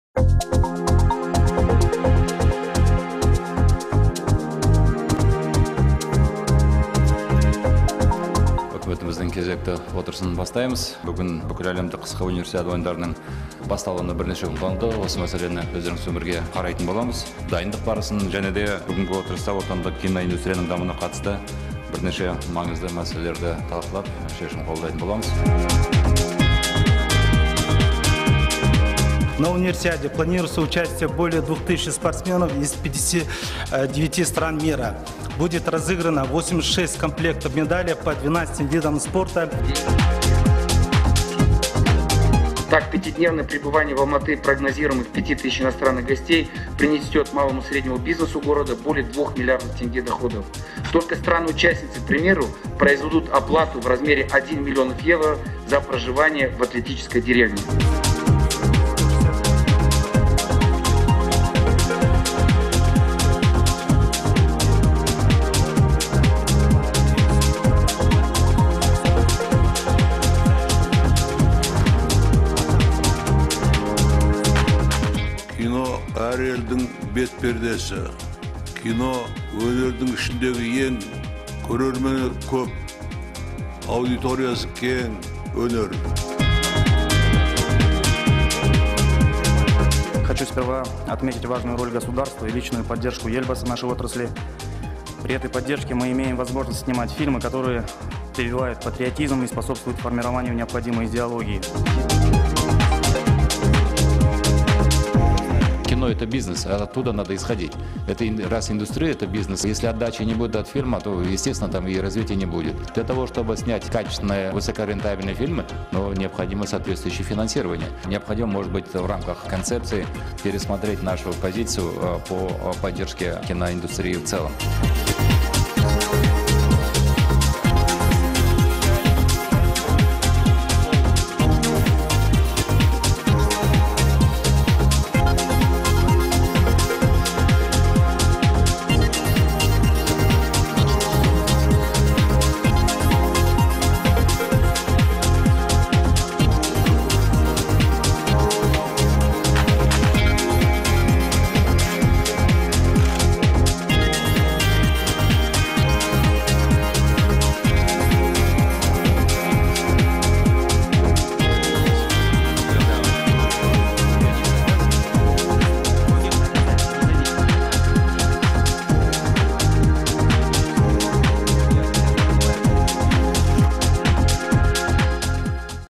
Видеодайджест: «Аптаның айшықты оқиғалары» №17
Осы аптада ҚР Премьер-Министрі Бақытжан Сағынтаевтың төрағалығымен Қазақстан Үкіметінің отырысы өтті. Онда екі мәселе қаралды: Алматы қаласында өтетін 28-ші Бүкіләлемдік қысқы Универсиада-2017-ге дайындық барысы және отандық кинематографияны дамытудың басым бағыттары.

Мәлім болғандай, 28-ші Бүкіләлемдік қысқы Универсиада-2017 биылғы 29 қаңтарда бастау алады. Алматыға әлемнің 59 елінен 2000-нан астам спортшы келеді деп күтілуде. Алматы қ. әкімі Б. Байбектің айтуынша, Универсиада кезінде бес мыңдай шетелдік қонақтың қалаға келуі шағын және орта бизнеске 2 млрд теңгеден астам кіріс әкеледі, спортшылар мен жаттықтырушылардың атлетикалық ауылда өмір сүруі үшін қатысушы елдер шамамен 1 млн еуро төлейтін болады. Министрлер мен Алматы және Астана қалаларының әкімдерінің баяндамаларын толығырақ сайтынан қараңыздар.

Отандық кинематографияны дамытудың басым бағыттары туралы мәселелер Қазақстанның көрнекті театр және кино қайраткерлері А. Әшімов, А. Сатаев, Р. Әбдірашев, Р. Нұғманов, А. Ибраев және т.б. қатысуымен талқыланды. Өз сөздерінде олар саланың әрі қарай жетілуіне қатысты пікірлерін білдіріп, кинематография мен киноиндустрия жобаларын қаржыландыруға байланысты арнайы заңның қабылдану қажеттігін айтты. Баяндамаларды толығырақ сайтынан қараңыздар.

Сондай-ақ, осы аптада Премьер-Министр Дүниежүзілік банктің Вице-президенті С. Мюллермен кездесті. Кездесу туралы толығырақ сайтынан оқыңыздар.

18 қаңтарда Б. Сағынтаев «Балқаш ЖЭС» жобасын іске асыру мәселелері бойынша кеңес өткізді.

Елдегі инвестициялық ахуалды әрі қарай жақсарту мәселелері бейсенбі күні Үкімет басшысының «Қазақстанның шетелдік инвесторлар кеңесі» Қауымдастығының басқарма төрағасы У. Вокуркамен кездесуі барысында талқыланды.

Осы күні Б. Сағынтаев еліміздің қаржы жүйесіндегі өзекті мәселелер жөнінде кеңес өткізді.

20 қаңтарда Премьер-Министр Қауымдар палатасының Сыртқы істер және халықаралық даму комитетінің төрағасы Роберт Нолттың басшылық етуімен, Канаданың парламенттік делегациясымен кездесті. Кездесу барысында тараптар Қазақстан мен Канаданың әлеуметтік-экономикалық дамуының түрлі салаларындағы әрі қарайғы екіжақты ынтымақтастық келешегін талқылады.

Сол күні Б. Сағынтаев Қазақстан Республикасындағы Нидерланды Корольдігінің Төтенше және Өкілетті елшісі Дирк Ян Коппен кездесу өткізді.

Сонымен қатар, жұма күні Үкімет басшысы Қазақстанның бизнес ахуалын одан әрі жақсарту мәселелері бойынша кеңес өткізді. Кеңес барысында Дүниежүзілік банктің «Doing Business» рейтингісіндегі еліміздің позицияларын жақсарту бойынша заңнамалық бастамалар талқыланды.

Сенбі күні Премьер-Министр Б. Сағынтаев Үкімет мүшелерімен әлеуметтік-экономикалық күн тәртібі бойынша жұмыс кеңесін өткізді.